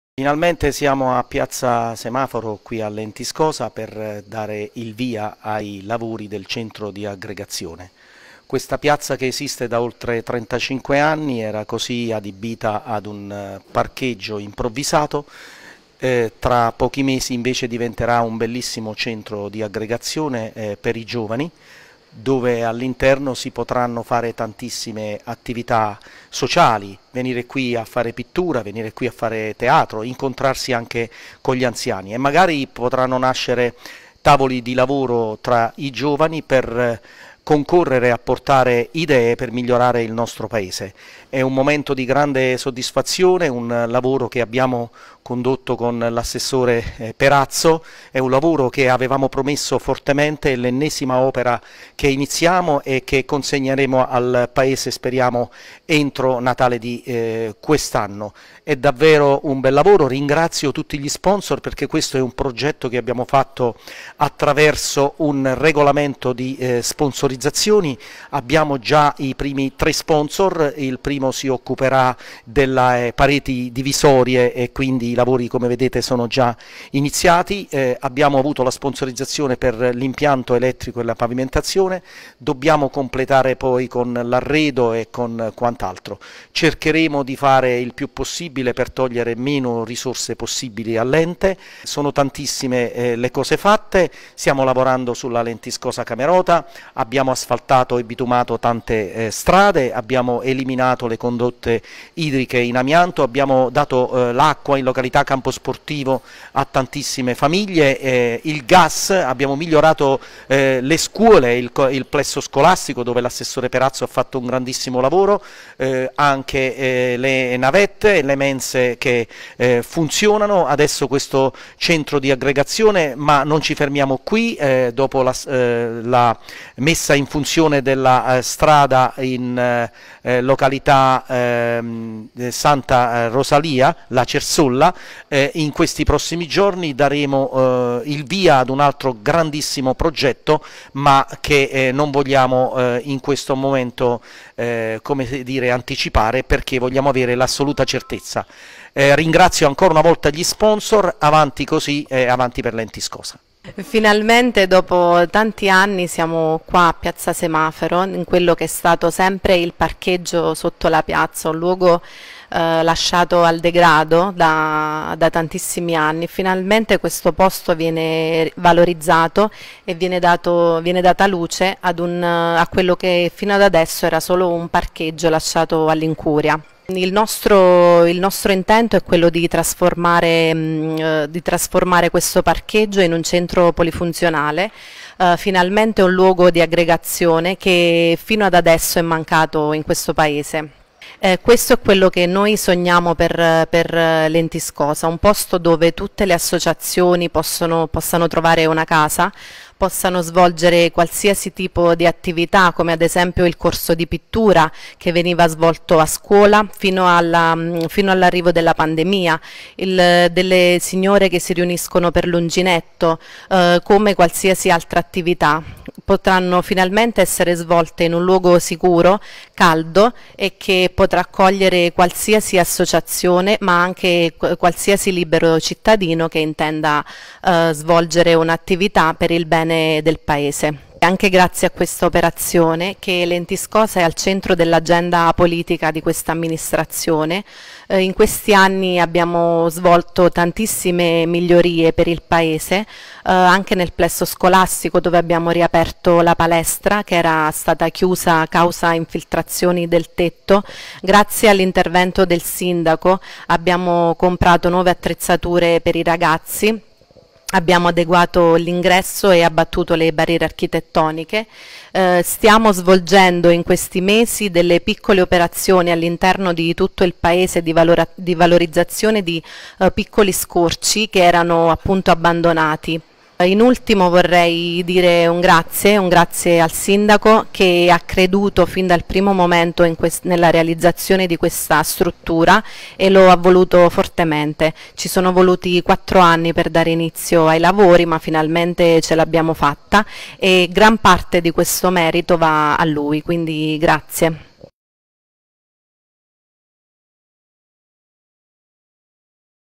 Camerota (SA) - Lentiscosa, partiti i lavori per il nuovo centro di aggregazione (06.07.21)
- Camerota (SA) - Lentiscosa, partiti i lavori per il nuovo centro di aggregazione
Presto lo spazio abbandonato sotto piazza Semaforo a Lentiscosa, sarà trasformato in un centro di aggregazione e consegnato alle associazioni e alla cittadinanza (06.07.21)